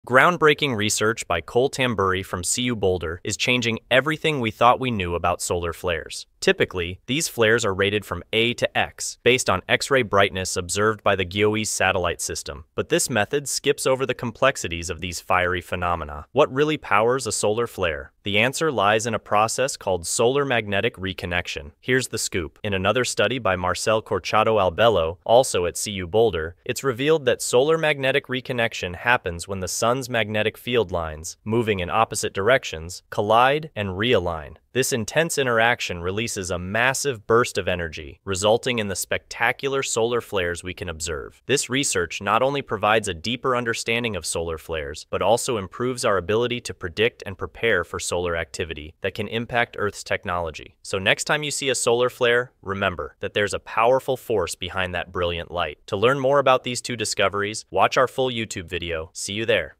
Breakthrough: Solar Flares and Solar Magnetic Reconnection Explained in Two New Studies
Dive into the fascinating world of solar flares with groundbreaking research from CU Boulder! Discover how solar magnetic reconnection fuels these fiery outbursts and how new classifications can help predict their impact on Earth. Perfect for anyone curious about how our sun's dramatic activities affect us all!